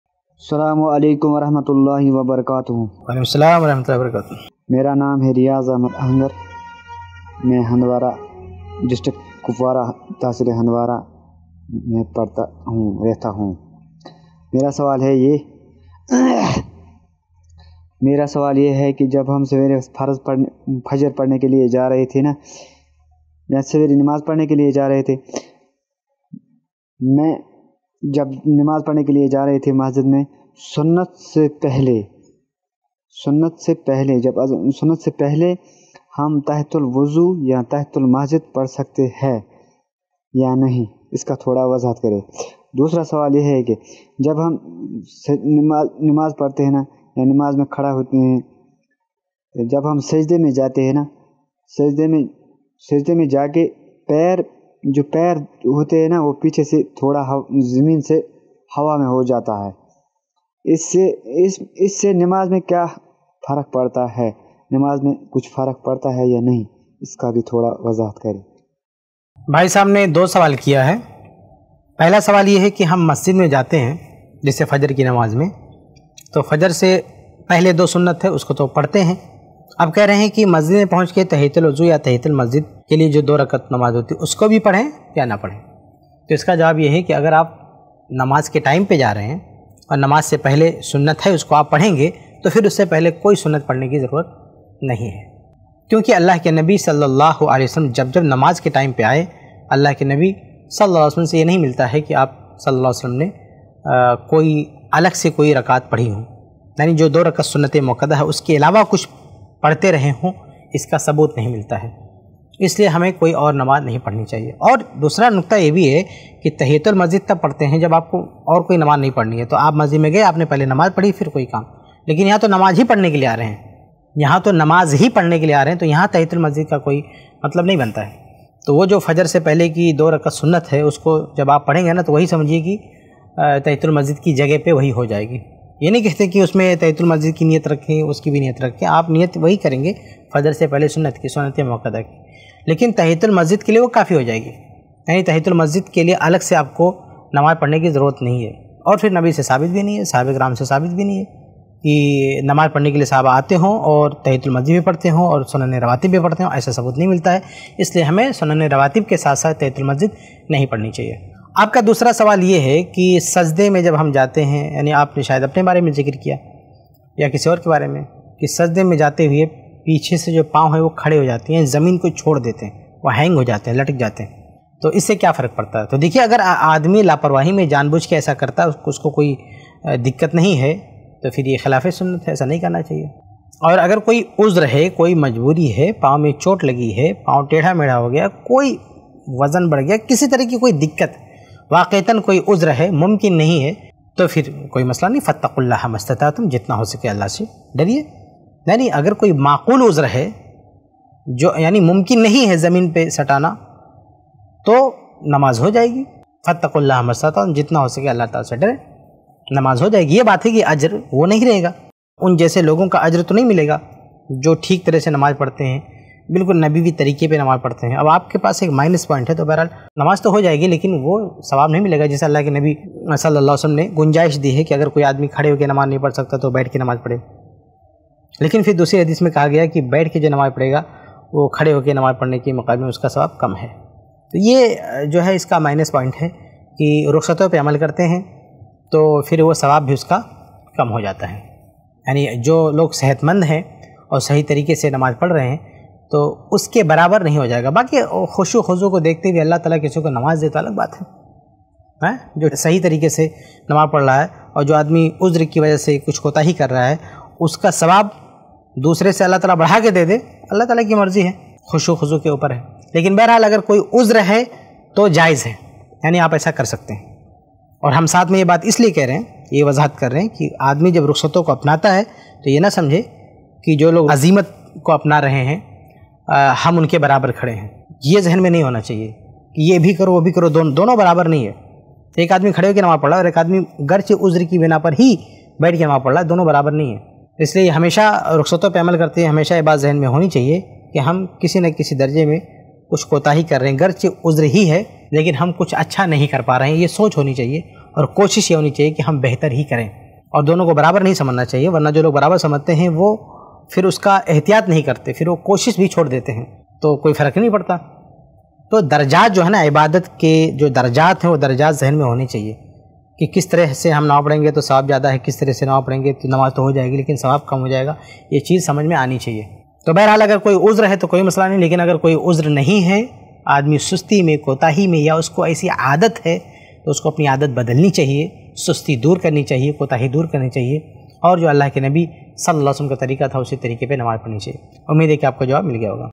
Fajr Ki 2 Rakat Sunnat Padhne Ke Baad Tahiyatul Masjid Padh Sakte Ya Nahi? by Shaikh Kifayatullah
Fajr Ki 2 Rakat Sunnat Padhne Ke Baad Tahiyatul Masjid Padh Sakte Ya Nahi? by Shaikh Kifayatullah Sanabili iPlus TV

UPI : donateilm@icici